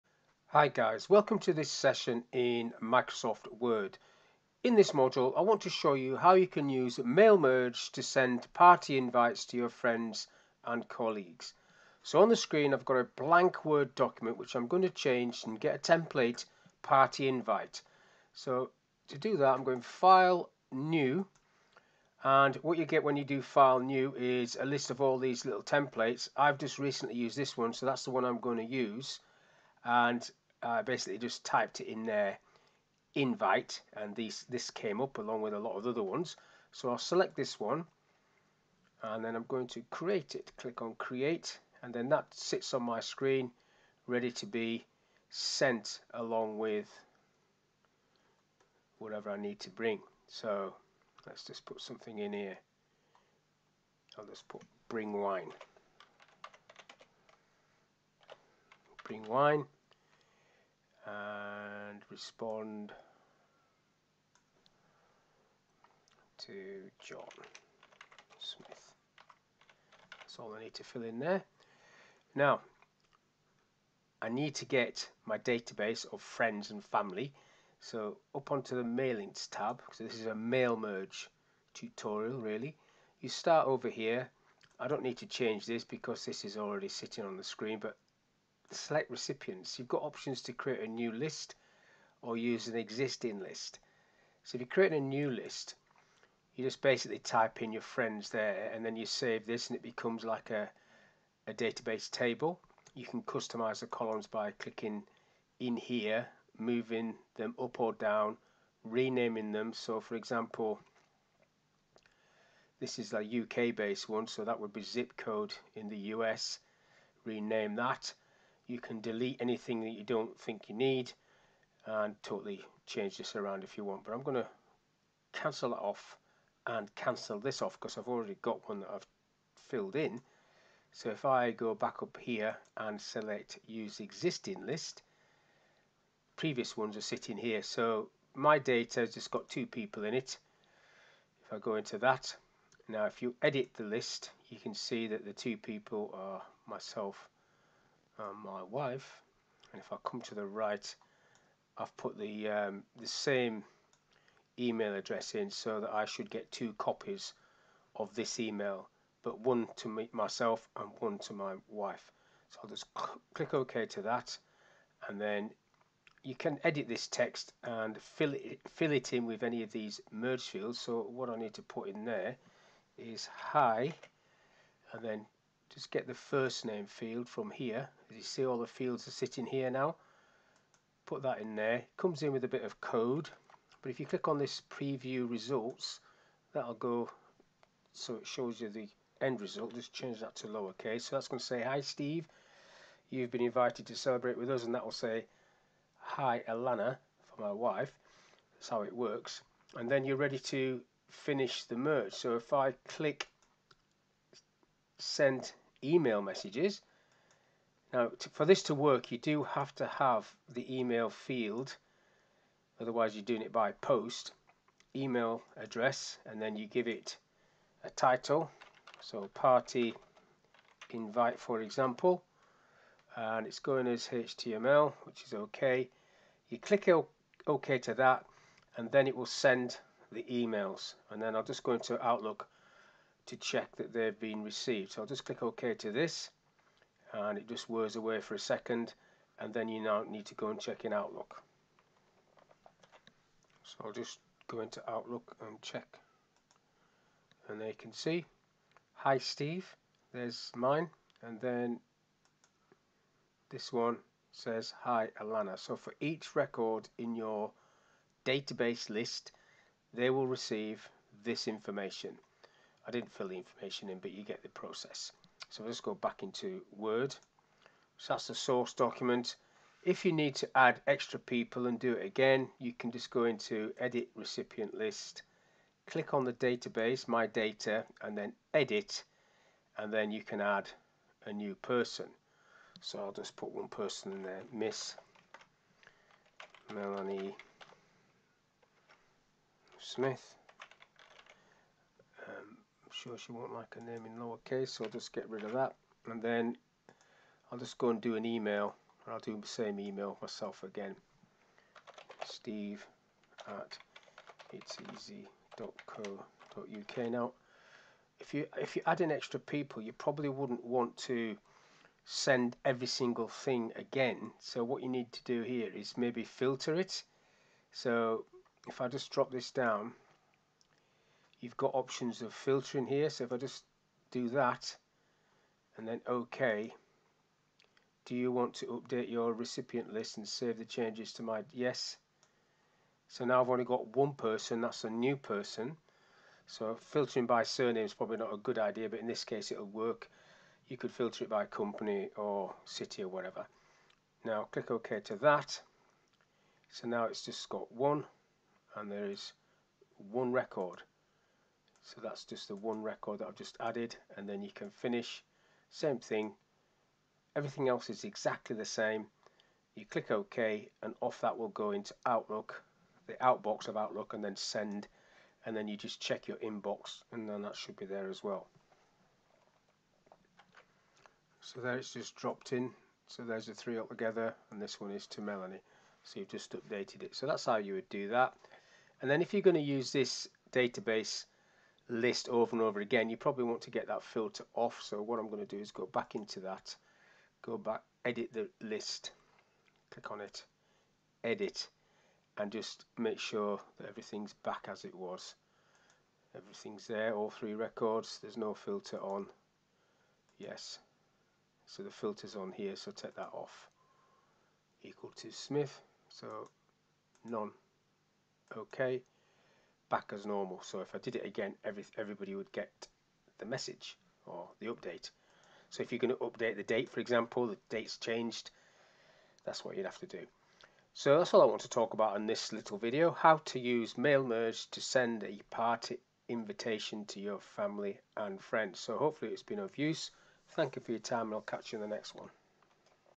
How to Mail merge a party invite using Microsoft Word
This is a Microsoft Word tutorial covering, how to mail merge a party invite using Microsoft Word. The tutor starts off by selecting a party invite template. The tutor demonstrates how to start the mail merge process and how to create a data source. The tutor uses an existing data source and inserts mail merge fields. The tutor then completes the mail merge and emails the invites to each person in the database. The tutor covers how the invites are received by displaying the emails in Outlook. Mail merge a party invite using Microsoft Word.